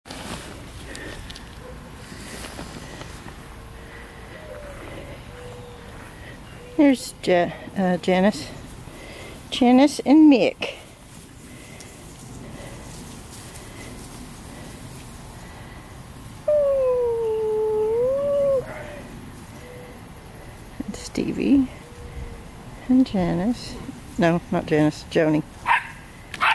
More of the Poppy/Tyler puppies. The lighter golden sable with the brown front leg is the boy Mick.